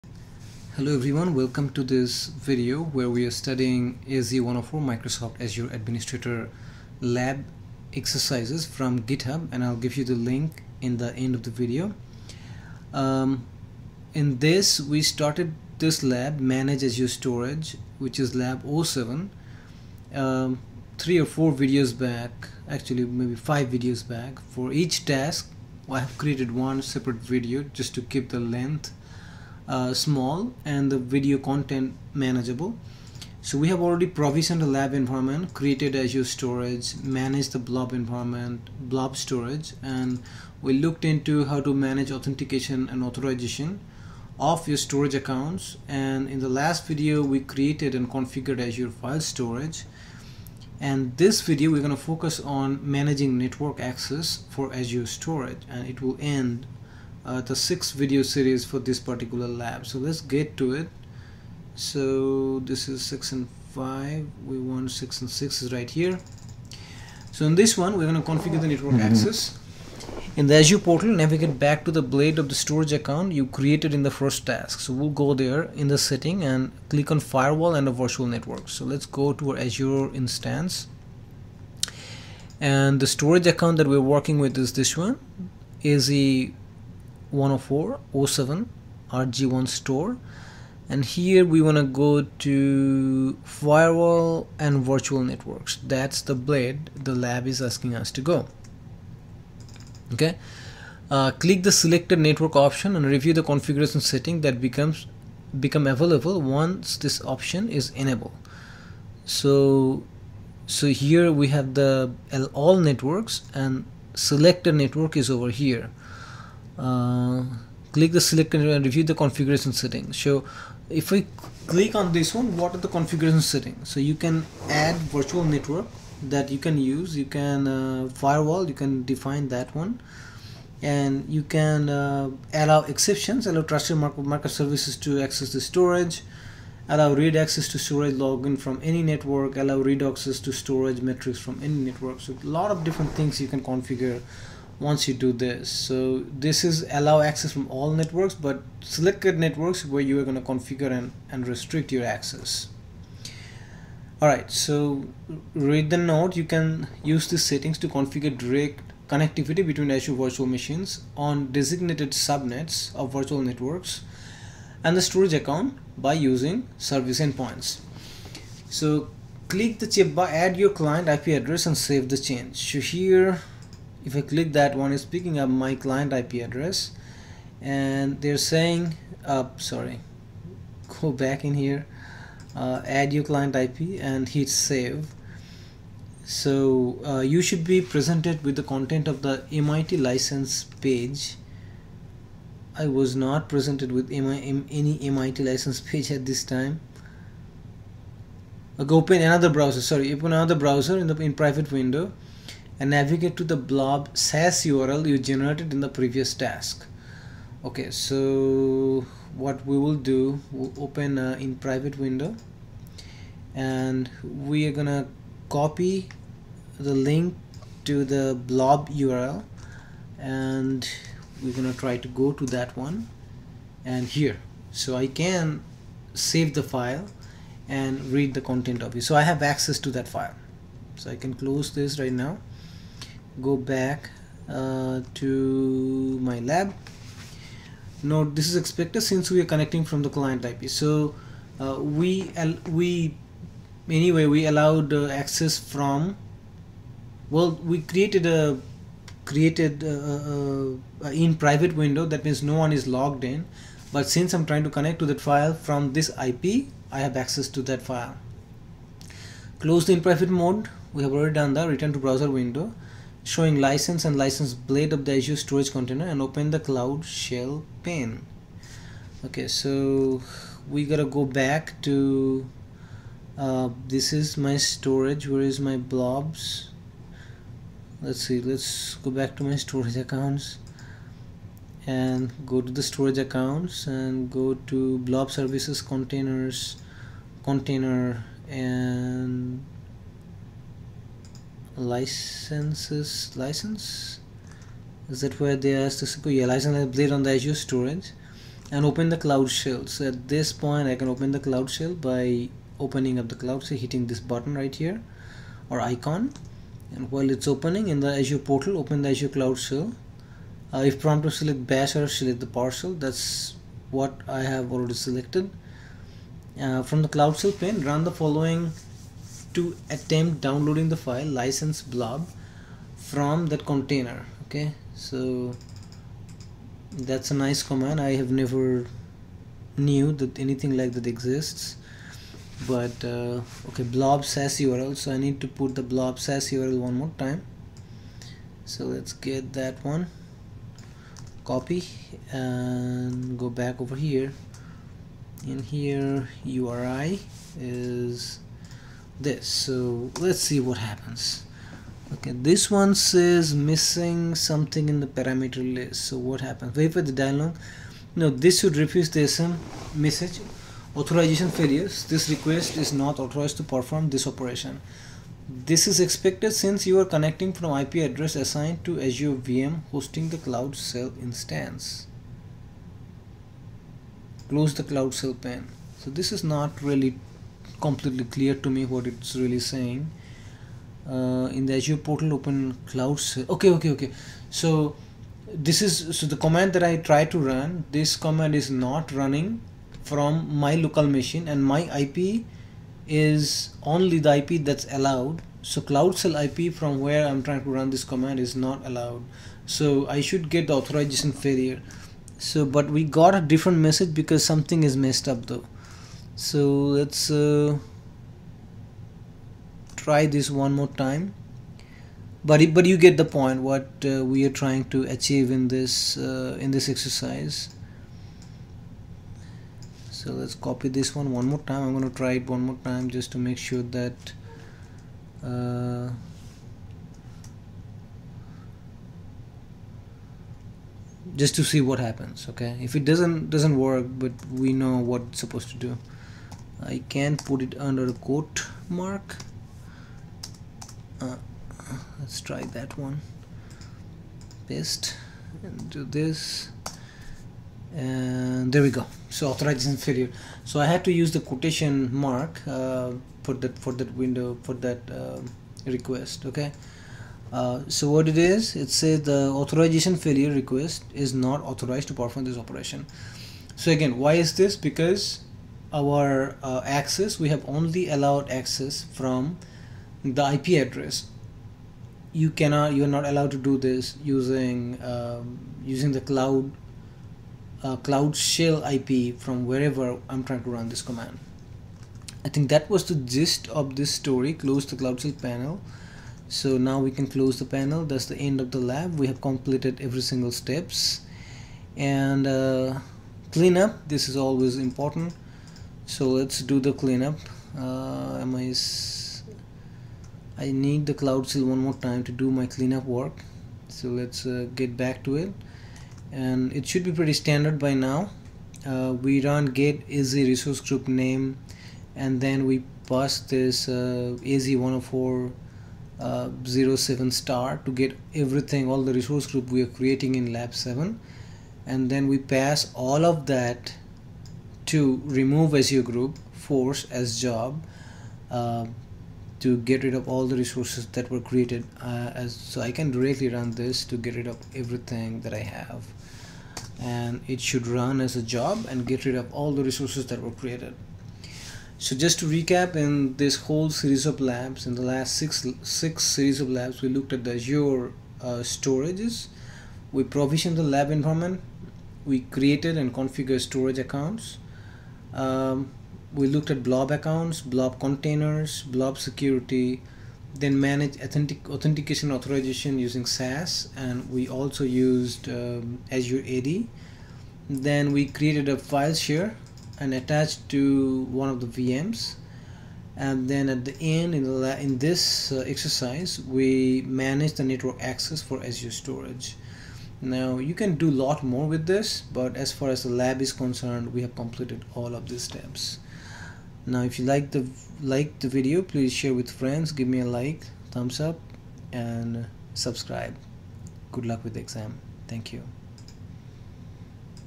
AZ 104 Lab 07 - Manage Azure Storage Task 6: Manage network access for Azure Storage - Azure Admin
Task covered in this video:
Task 6: Manage network access for Azure Storage

I am making these set of videos as I prepare myself for the AZ 104 Azure administrator exam.
Make sure you complete all of the labs from Gitlab to get hands on real life experience on Azure Administration.